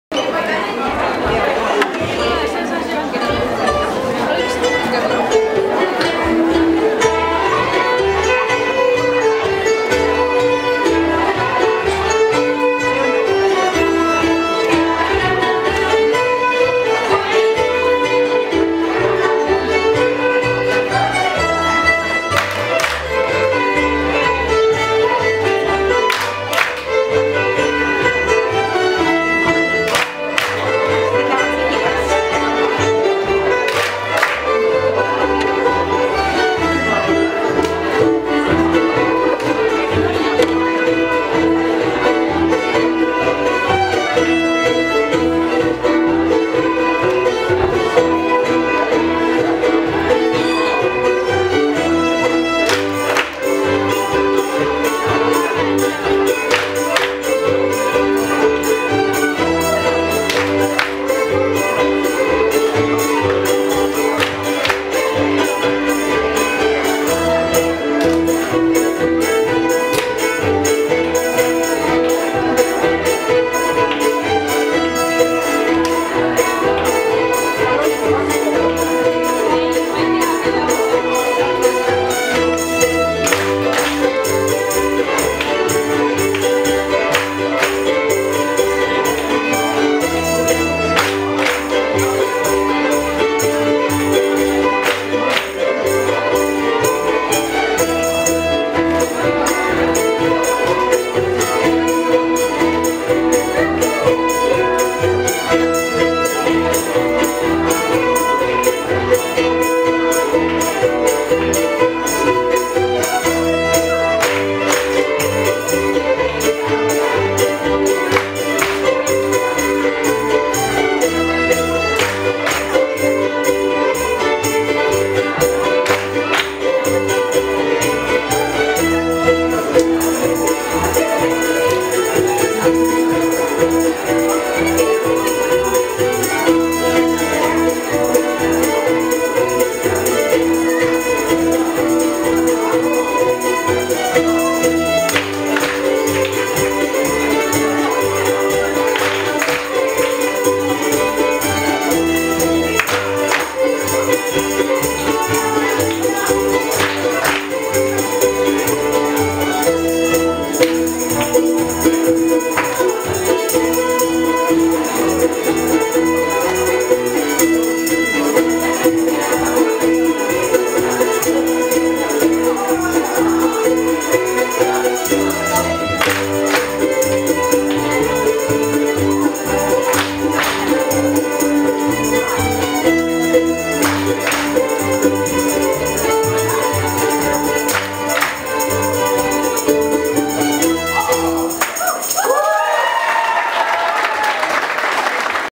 Kohaanuška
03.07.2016, V Laste Oma Folk; Salutaguse Muusikaait; tantsu juhendas Hans Mihkel Vares